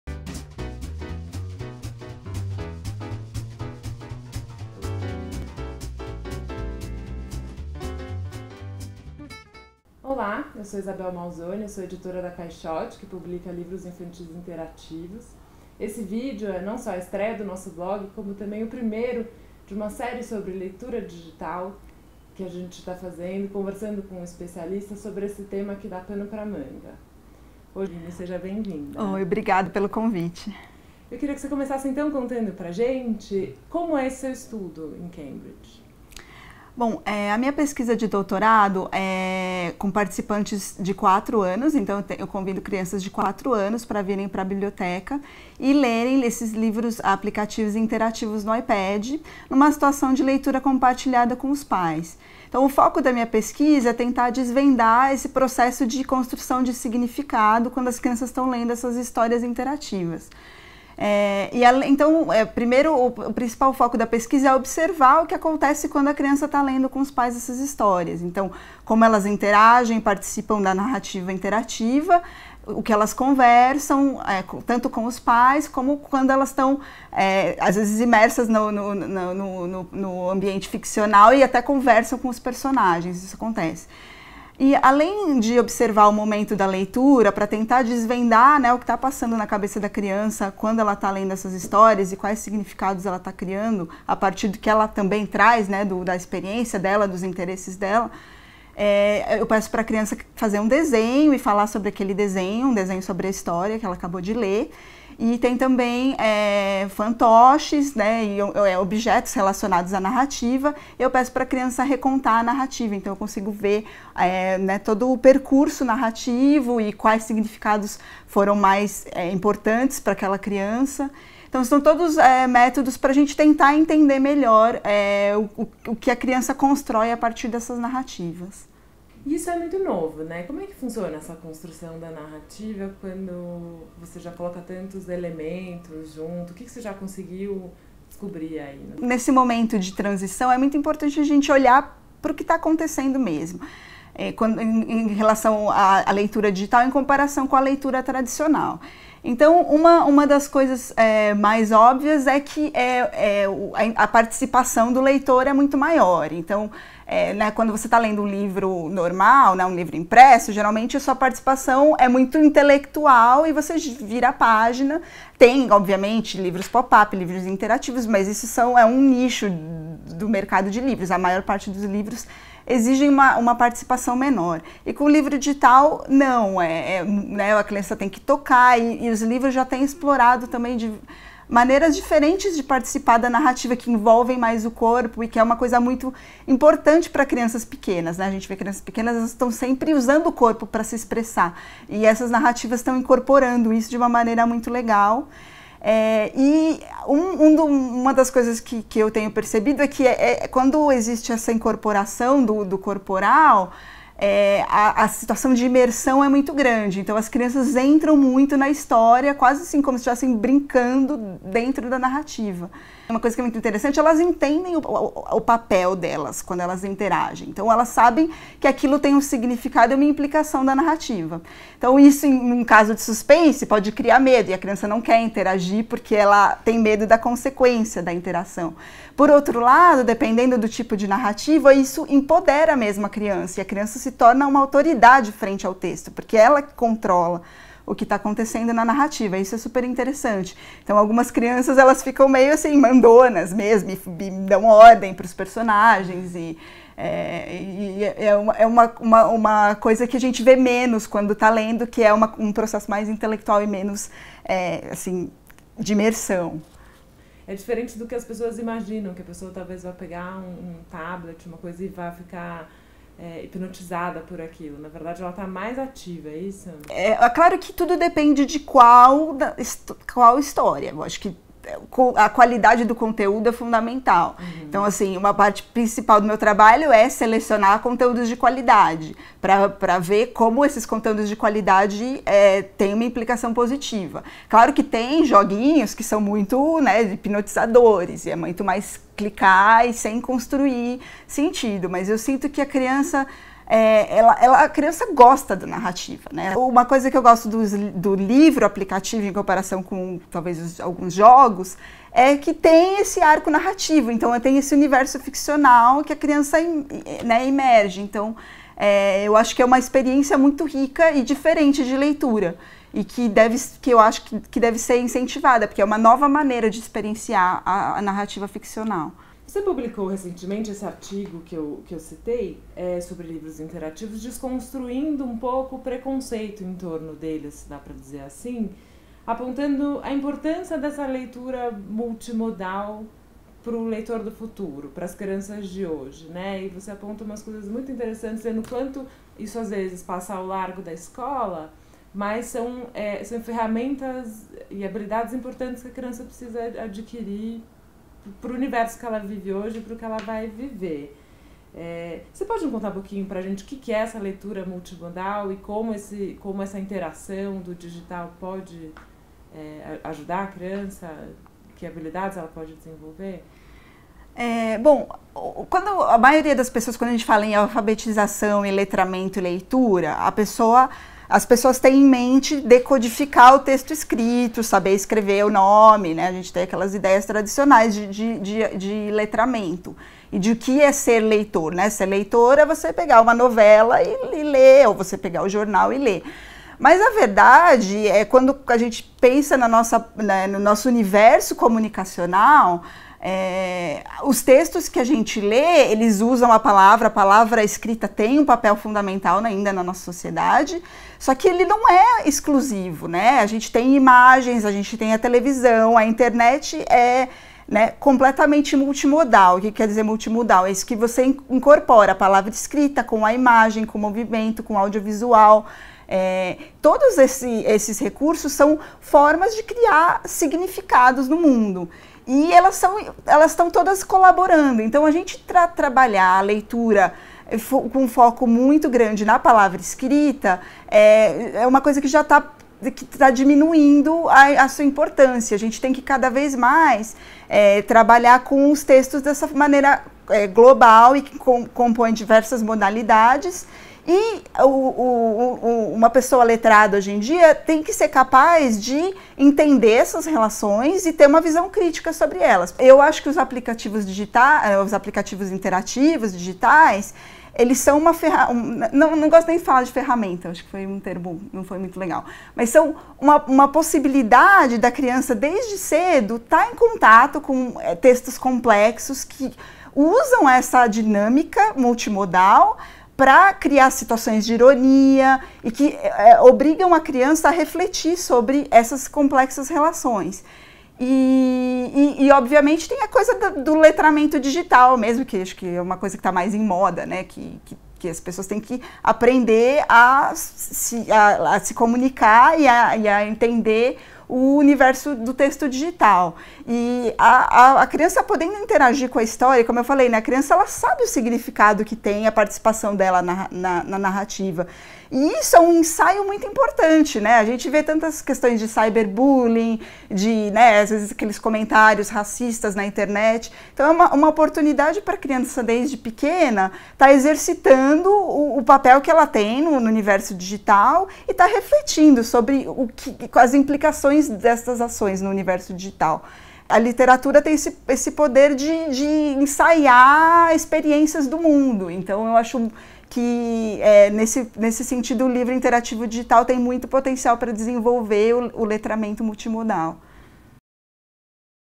Livros infantis interativos e as novas formas de ler - parte 1
Esse vídeo é a primeira parte da entrevista com Aline Frederico, PHD em leitura digital pela Universidade de Cambridge. Ela fala sobre o papel dos livros interativos digitais no letramento das crianças e sobre o leitor do futuro.
Quer saber mais? Visita nosso blog